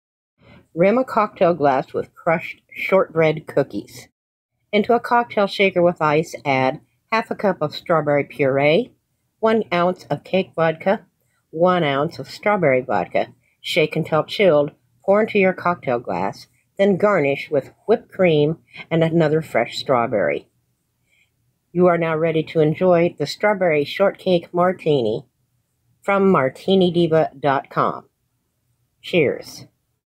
Strawberry Shortcake Martini Cocktail Recipe Video
An animated graphic art illustration by PopArtDiva of the Cake Vodka, Strawberry Vodka and Strawberry Puree STRAWBERRY SHORTCAKE MARTINI recipe from The Martini Diva.